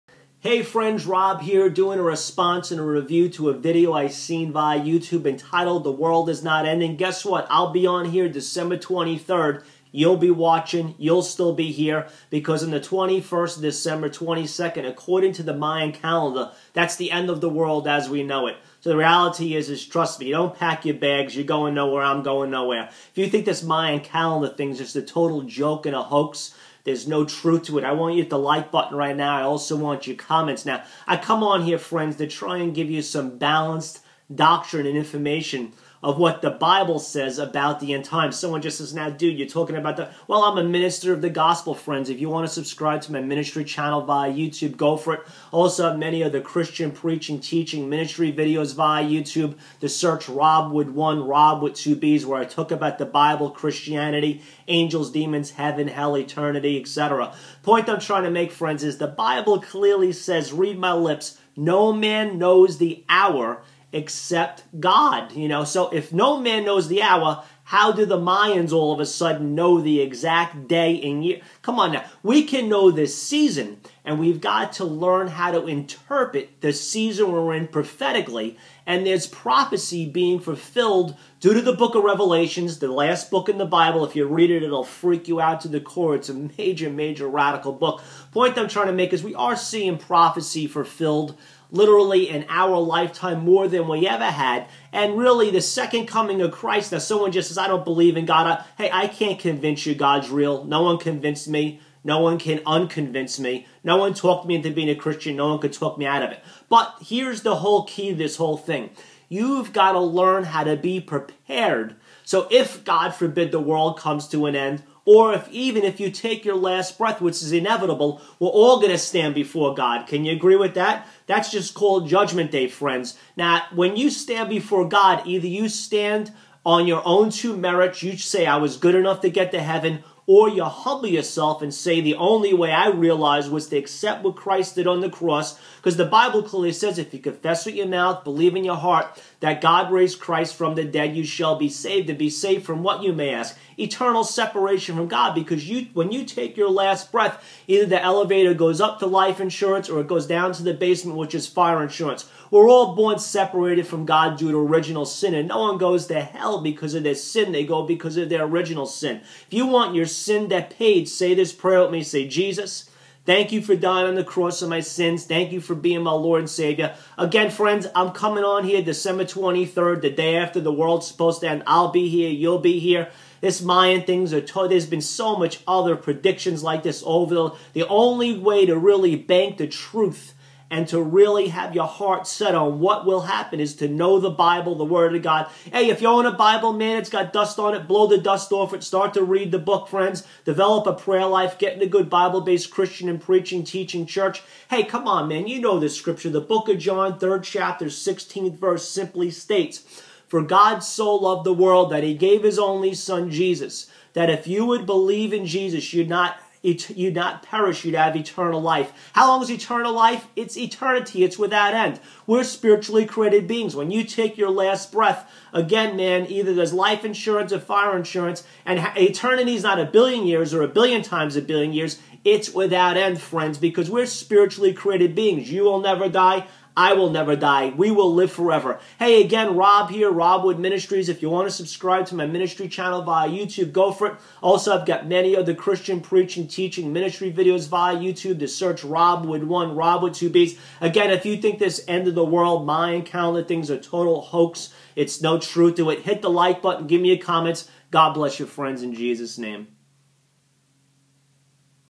THE WORLD IS (not) ENDING!?!:Re
Robb shares and asks if   you feel THE WORLD IS REALLY ENDING!?!